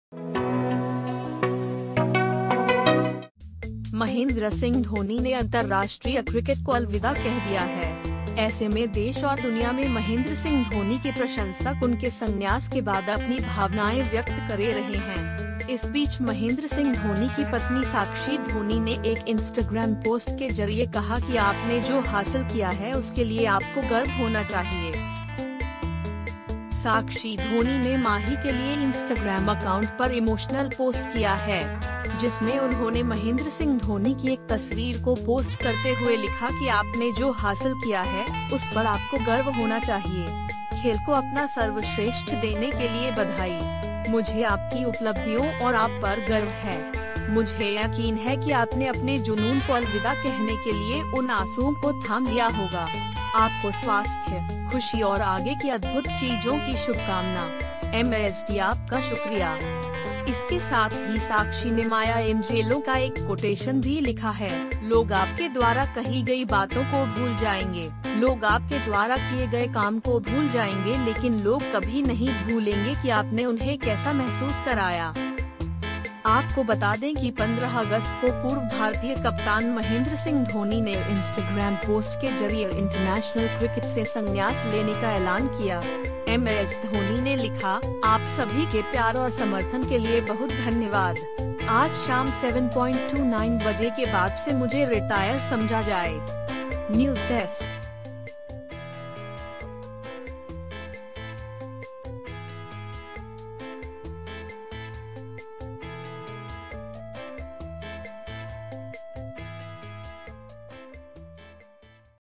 Sakshi Singh ने Dhoni के लिए लिखा इमोशनल पोस्ट,बोली-आपने जो हासिल किया उसके लिए आपको गर्व होना चाहिए
महेंद्र सिंह धोनी ने अंतरराष्ट्रीय क्रिकेट को अलविदा कह दिया है. ऐसे में देश और दुनिया में महेंद्र सिंह धोनी के प्रशंसक उनके संन्यास के बाद अपनी भावनाएं व्यक्त करे रहे हैं.